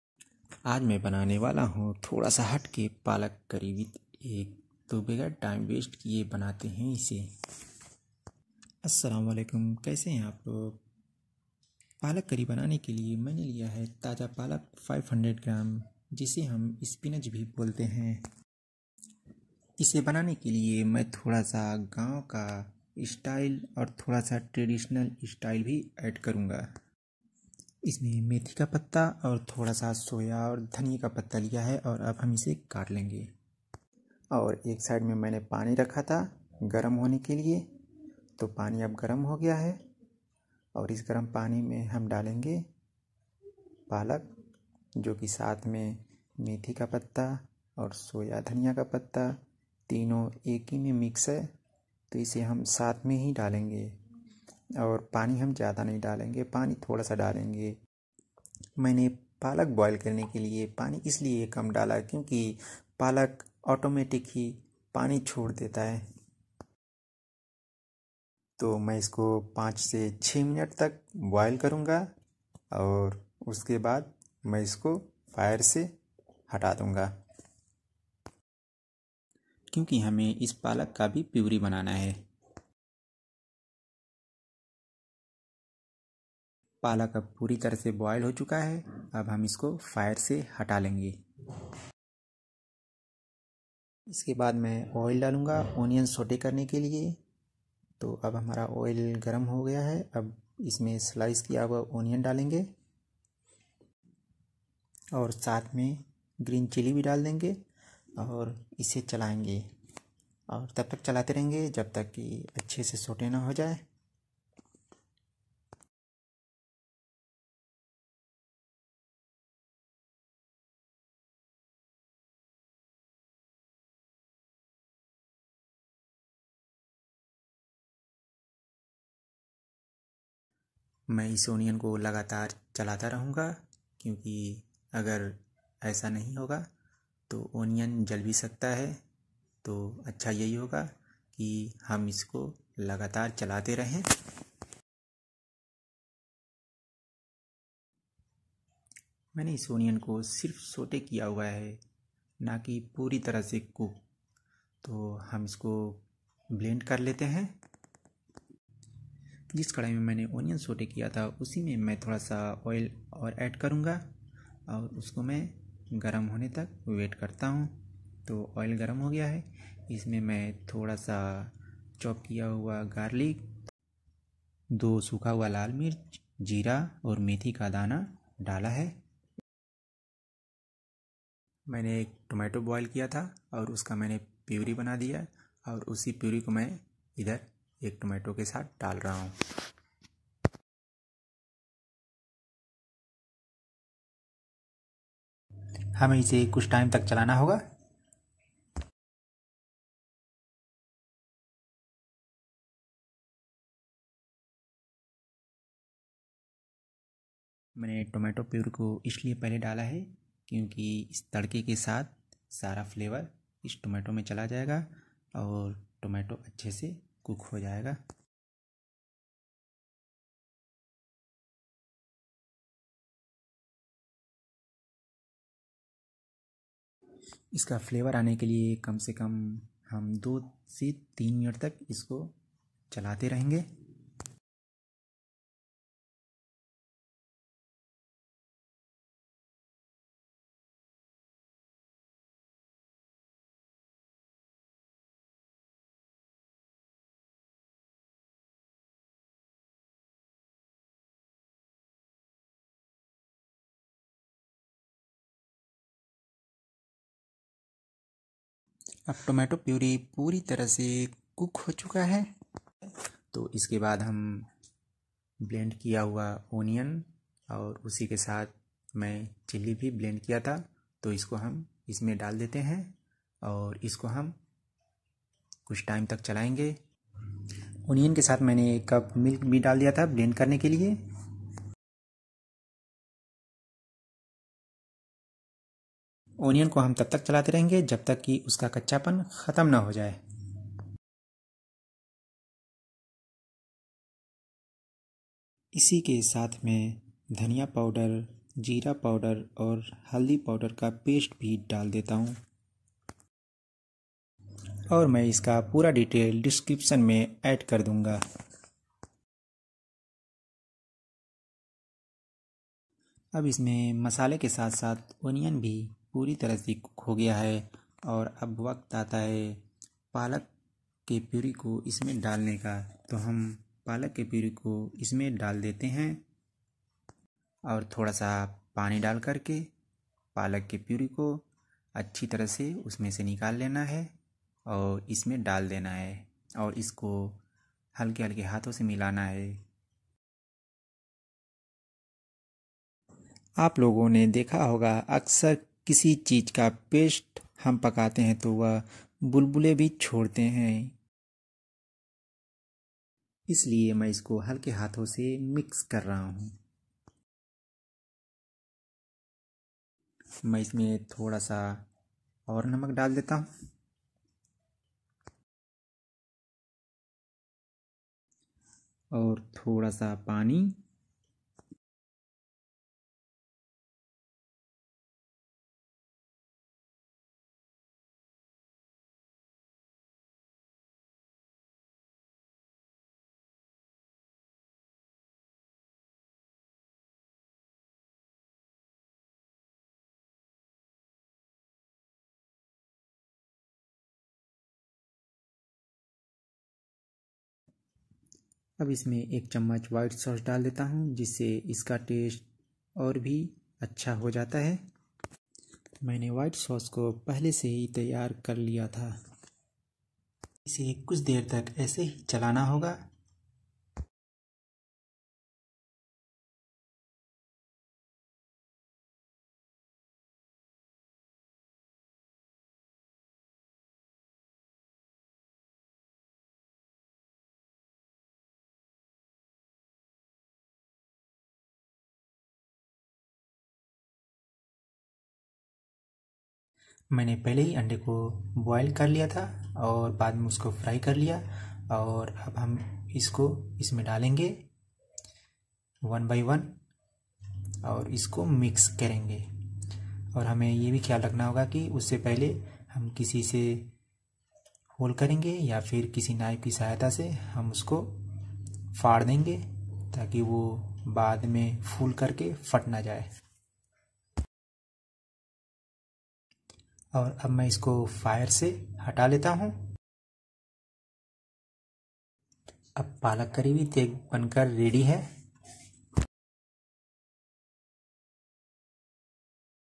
Palak Curry|How To Make Palak Curry With Egg|Spinach Cury
palak curry banyen  white sauce ke saath.alag test me.
      Maine is Video mein palak curry with egg banane ka tarika bataya hai ki kaise banate hain.
     app humein comment mein feedback zaroor dein.

Isme Maine
   half tbls coriander powder
   half tbls cumin powder
   turmeric powder 5gm
 pest bana liyaa